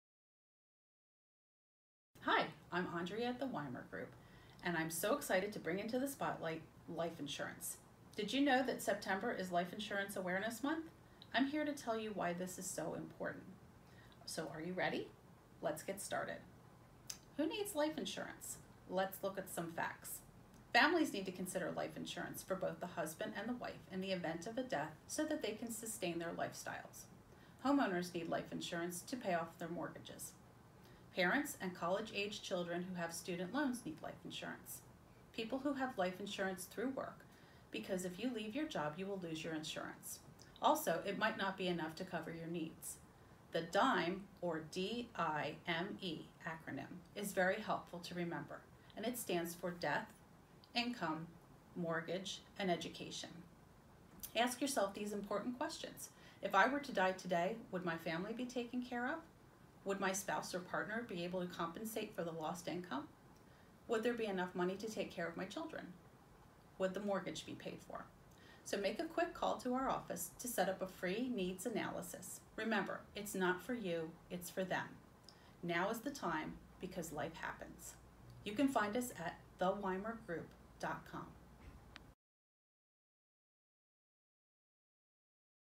Who Needs Life Insurance, and why?
September is Life Insurance Awareness Month.  We would like to spotlight a number of reasons why you need life insurance.  Watch this informative video.  We can help you decide what coverage is right for you and your family, that also fits your budget.